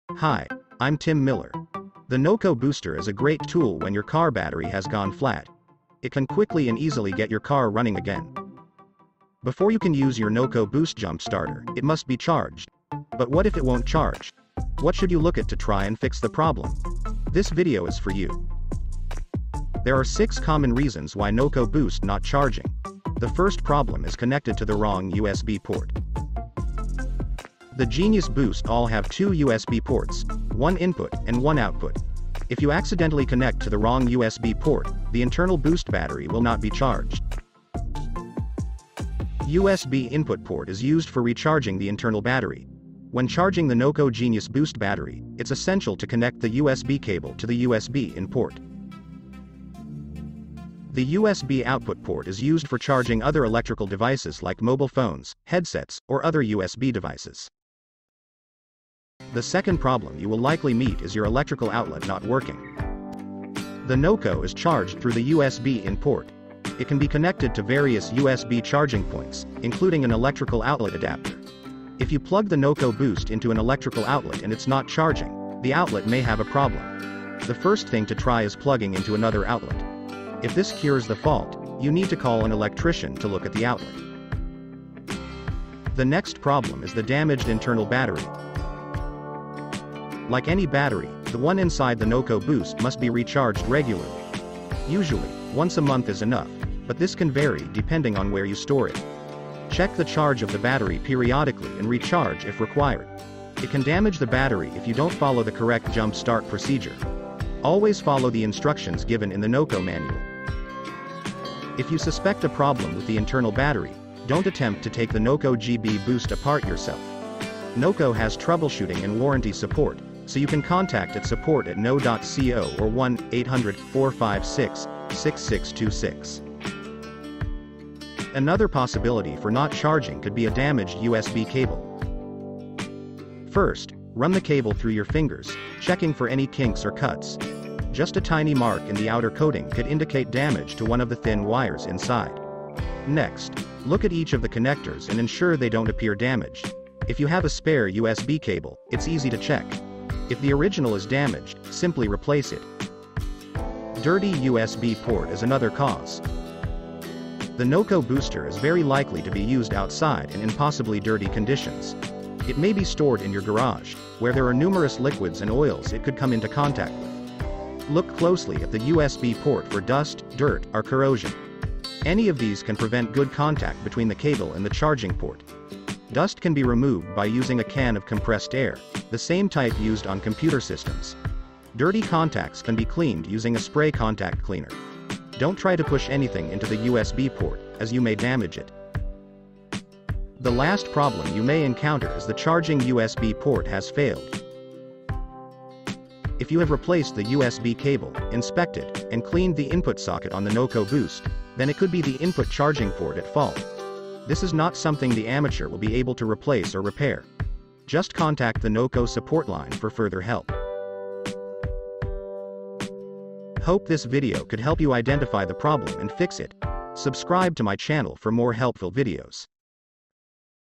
Noco Genius Boost Not Charging? 6 Problems & Solutions
What should we do if the Noco jump starter won’t charge? What should you look at to try and fix the problem?

Check the 6 common problems below:

1. Connected To The Wrong USB Port
2. Electrical Outlet Not Working
3. The Internal Battery Is Damaged
4. Damaged USB Charging Cable
5. Dirty USB Port
6. Charging USB Port Has Failed